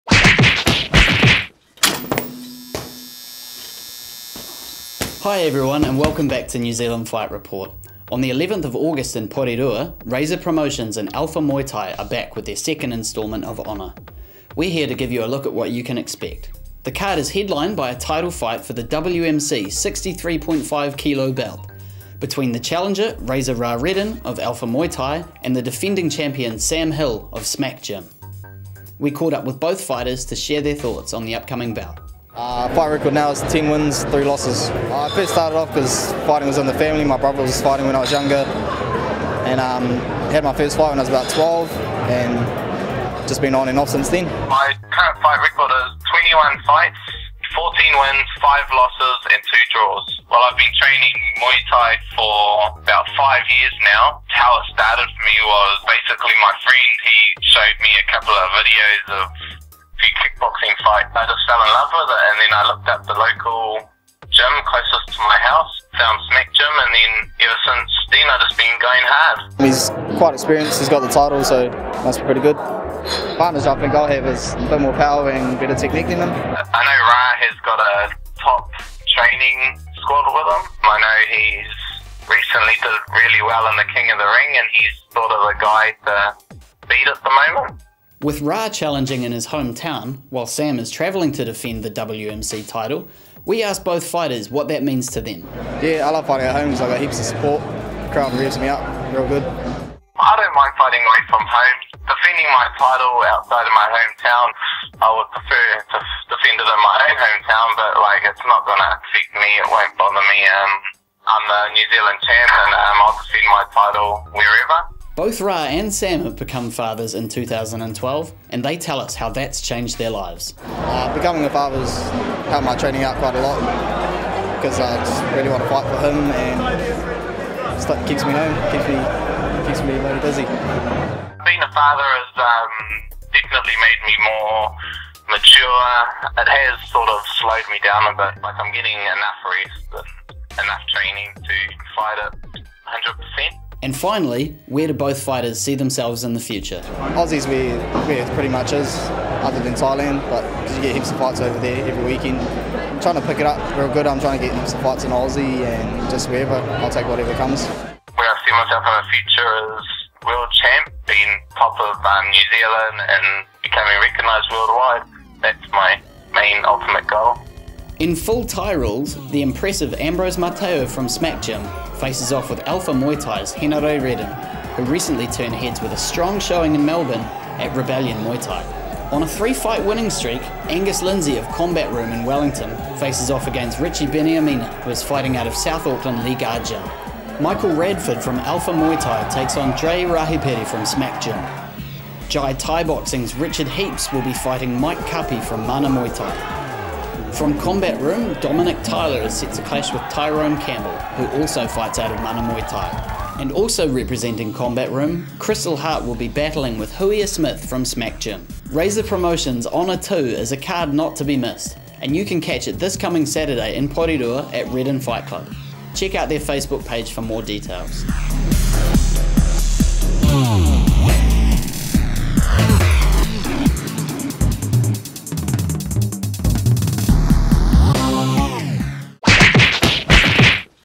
New Zealand Fight Report: Honour II - August 11, June 2012
Copyright Pound For Pound Pictures 2012

Jason previews the card for Razor Promotions Honour II, this Saturday night at Redden Fight Club. The team also interviews Ra Redden and Sam Hill regarding their 63.5kg WMC Title Clash, which headlines the fight night.

Produced by Jason Gascoigne, Bailey Palmer, SJ McCann & Rhydian Thomas

Studio Director: Brent Whitwell

Edited by Bailey Palmer

Graphics by David Duxfield

Thanks to Kevin Dick, Angela Clark, Ra Redden, Sam Hill, Victoria Nansen, SMAC, Alpha Muay Thai, Mark Hampton, Tim Simpson, Tim Hill, Ben Whitney and Avalon Studios.